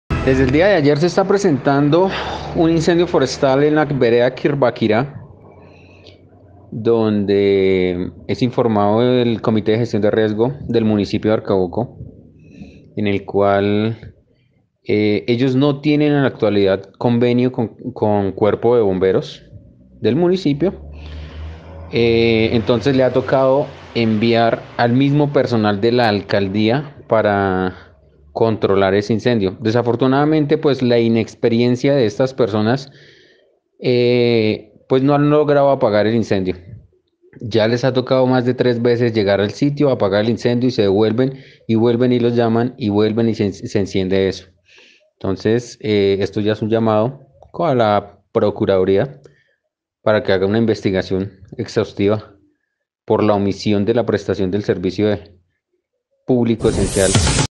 Incendio en Arcabuco se ha reactivado varias ocasiones, Alcaldía aun no firma convenio de Bomberos
Incendio en Arcabuco se ha reactivado en varias ocasiones, la alcaldía aun no ha firmado convenio con el Cuerpo de Bomberos.